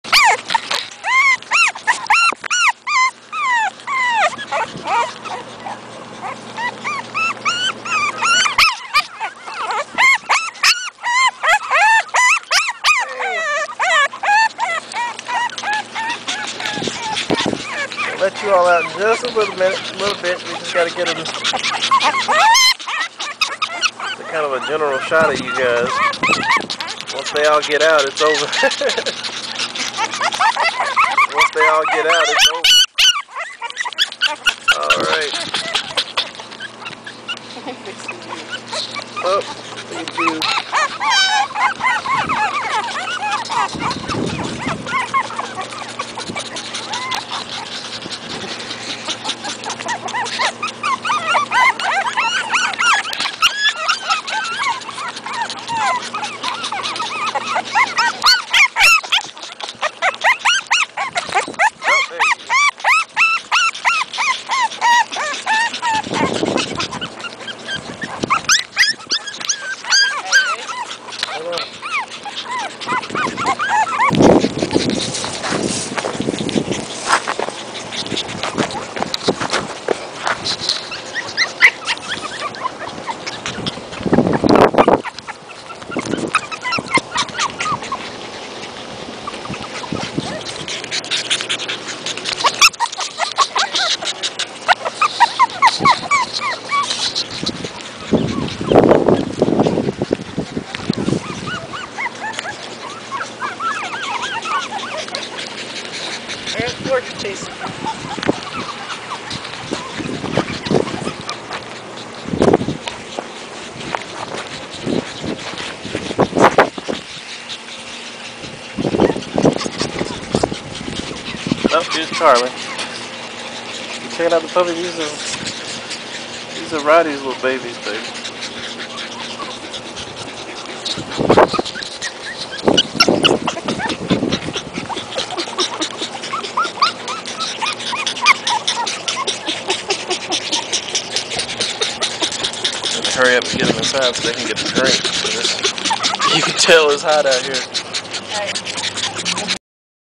BUGG (PUG BOSTON TERRIER MIX) PUPS FOR SALE IN MIDLAND TEXAS
Here's a vid of our Bugg pups for sale! They are Seven weeks, eat solid foods, drink on their own. We don't have any of the tan/fawn left.  We have 2 females and 3 males left. These little guys are beyond adorable! Private message us with SERIOUS inquiries only.